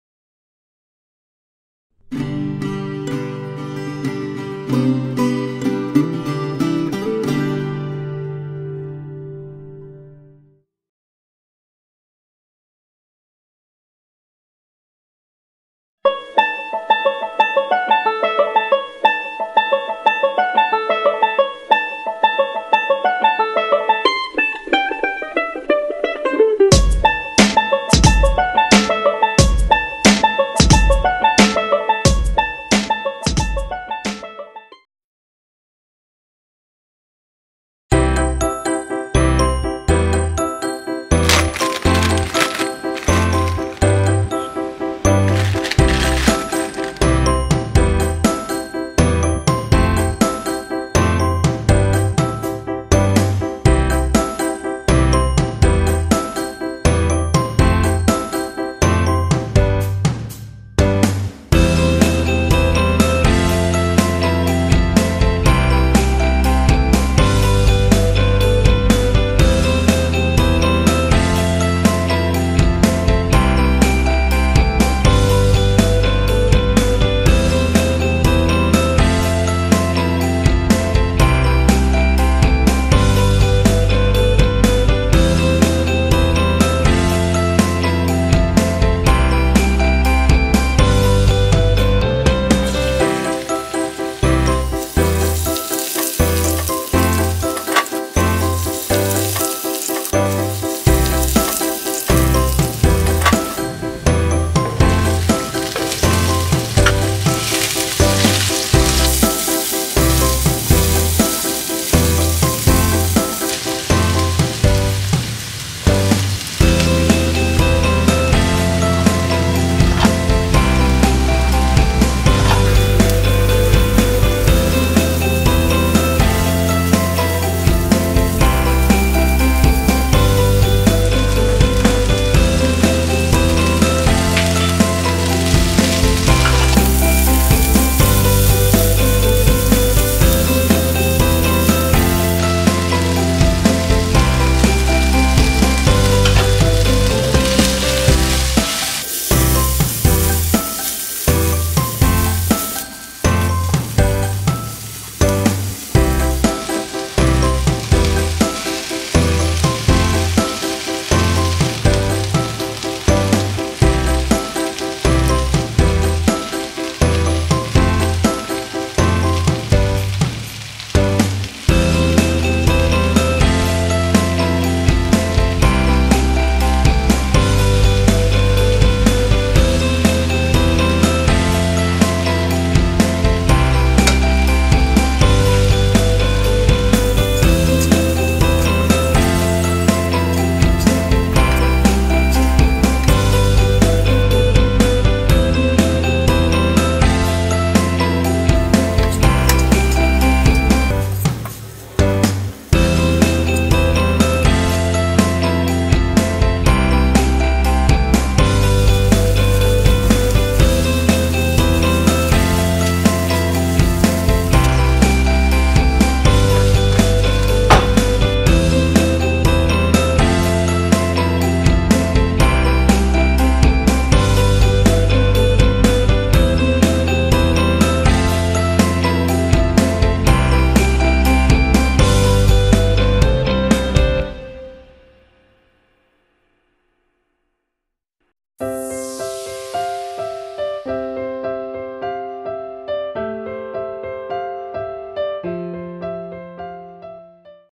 CHOW CHOW FRY Recipe - Easy & Quick Breakfast Recipe | Veg. Instant Noodles Wai Wai
CHOW CHOW FRy (Instant Noodles) Recipe is one of the most popular dish in nepal, easy and quick to make. Anyone can make it.
Watch this video to find how to make delicious chow chow fry recipe.
Welcome to "Learn to Cook with me CHANNEL"
Don´t forget... If you like this recipe... Leave a comment or Thumbs up ;) Thank you.